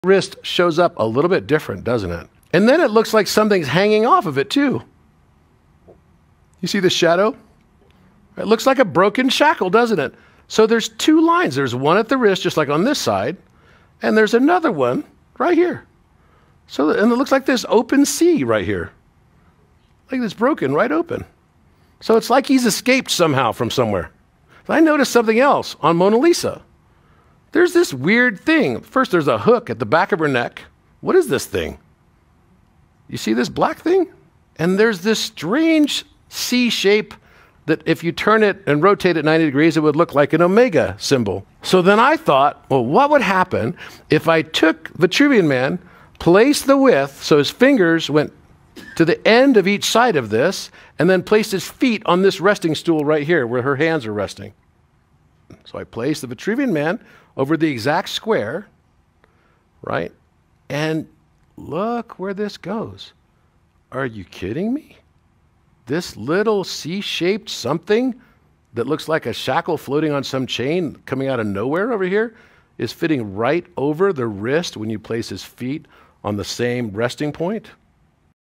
Decrypting the Vitruvian Man + Mona Lisa: The Alchemical Wedding
Leonardo da Vinci was an alchemist and Rosicrucian who encrypted many alchemical symbols into his artwork. He famously encrypted several of his masterpiece paintings particularly after 1486 (the Vitruvian Man was first recorded in 1487 following his "Missing Years" in Italy from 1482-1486).

Are the Leonardo Masterpieces meant to be viewed as part of a larger 'mosaic' encryption?

Film via GaiaSphere 'The Divine Encryption' Event in Boulder, Colorado, USA, 2022.